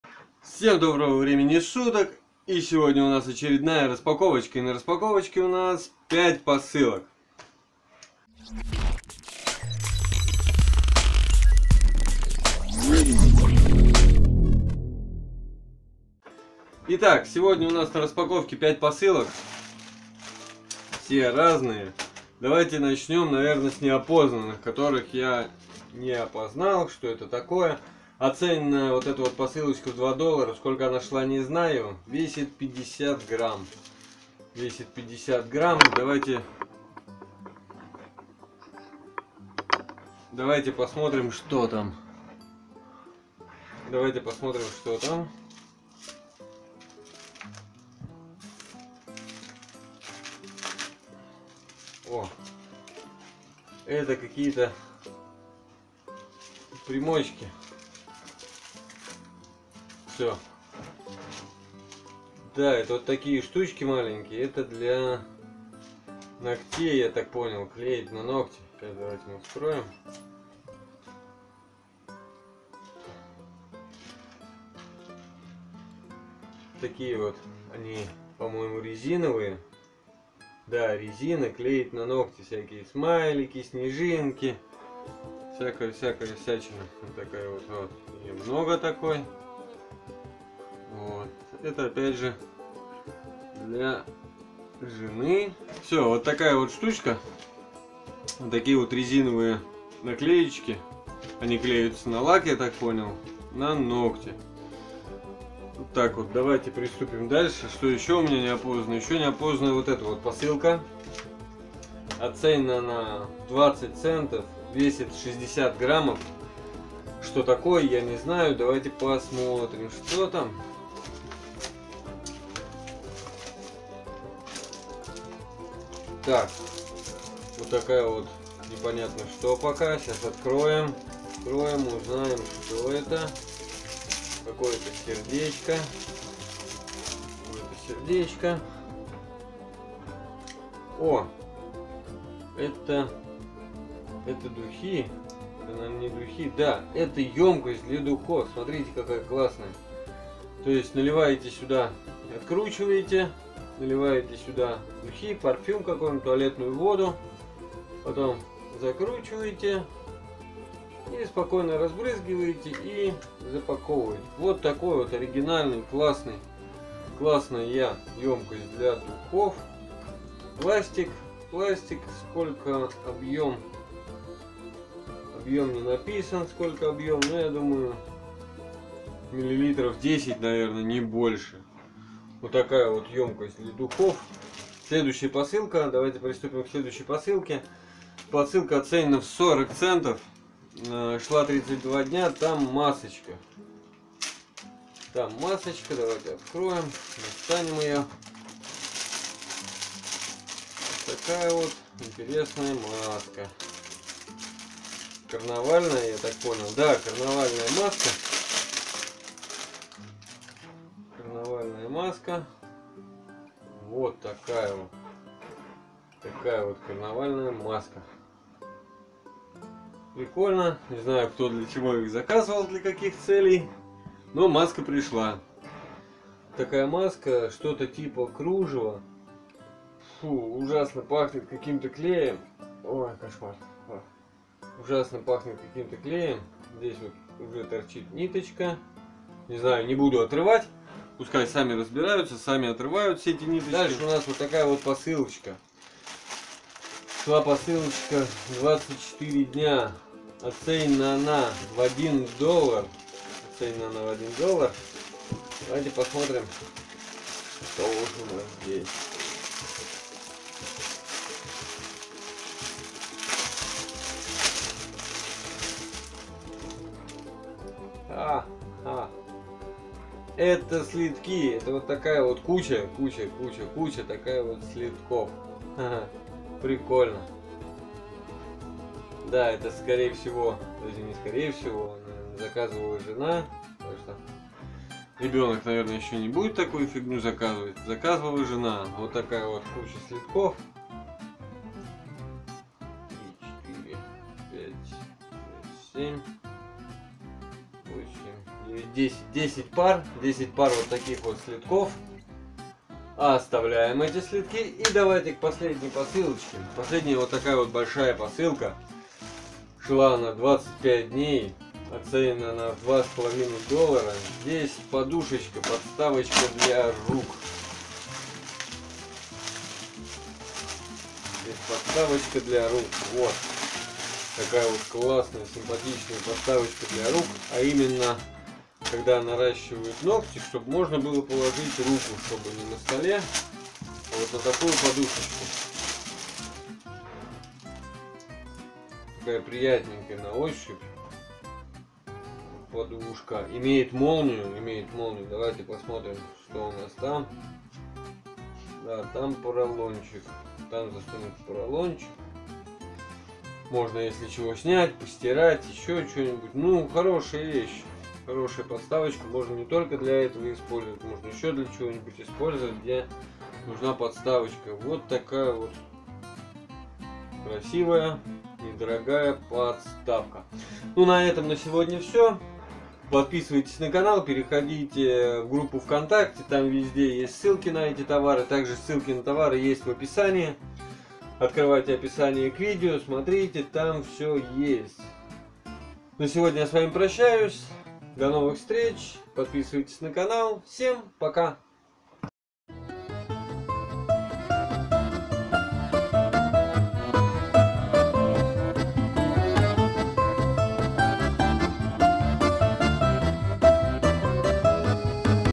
Новая распаковка, Пять интересных посылок из Китая для девушек.
Разверни описание и узнай больше
  Пришли сегодня пять очень интересных для девушек посылок.
1- Наклейки_на_ногти, похоже что из какой то пористой резины. Не пахнут , выглядят симпатично.
2 - Флакон_для_духов. Пластмасса но выглядит прикольно, Подойдёт для духов , и прочих жидкостей
4 - Следки - Без комментариев. Жена сказала что очень прочные и хорошо носятся .
5 -Подставка_под_руки. Опять же по словам жены, очень удобно. Подкладывает под руки когда кому то красит ногти

И всё это вы можете увидеть на канале

Помощь каналу:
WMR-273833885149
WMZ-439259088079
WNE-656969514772